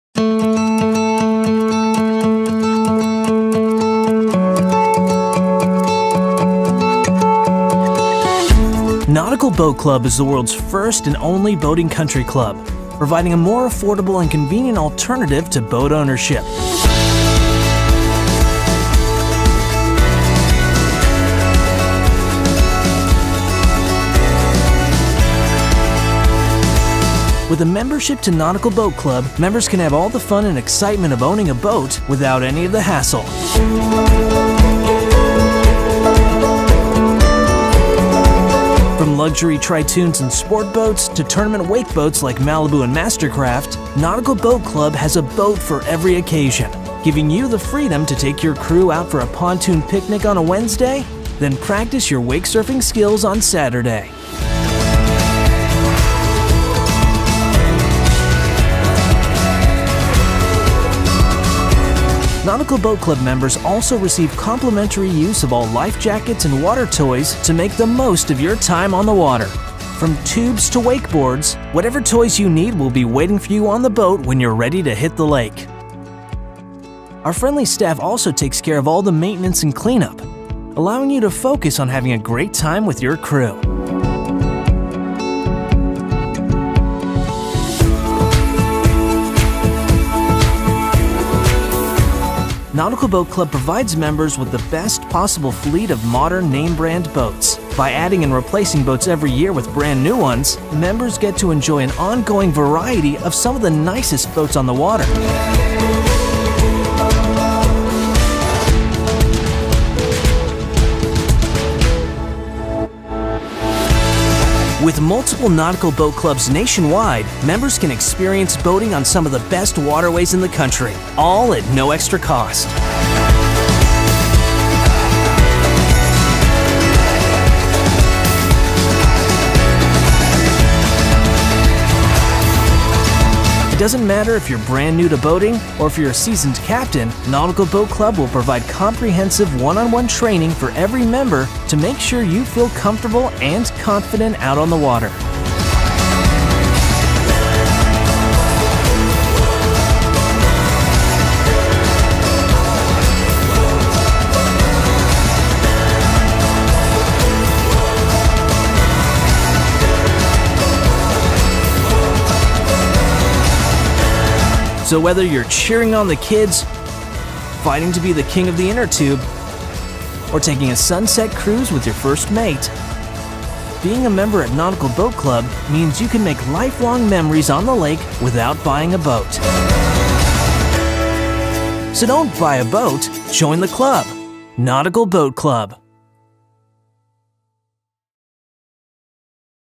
Nautical Boat Club - See How It Works - Club Overview
Don't Buy A Boat® - Join The Club! 1/3 The Cost Of Owning A Boat & None Of The Hassles.  A Fleet Of New Boats, Free Water Toys And Online Reservations For Fun On The Water All Year Long.  See How It Works And Join Charleston's Only - Boating Country Club®. Get Started Today!